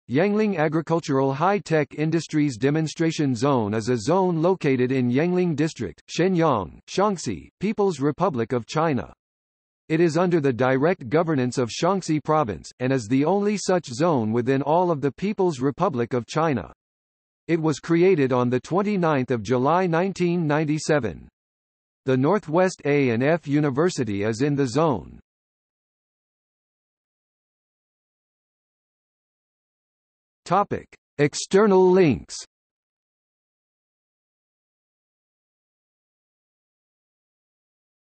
Yangling Agricultural Hi-tech Industries Demonstration Zone | Wikipedia audio article
00:00:26 External links

- Socrates

SUMMARY
Yangling Agricultural Hi-tech Industries Demonstration Zone  is a zone located in Yangling District, Xianyang, Shaanxi, People's Republic of China. It is under the direct governance of Shaanxi Province, and is the only such zone within all of the People's Republic of China. It was created on 29 July 1997.
The Northwest A&F University is in the zone.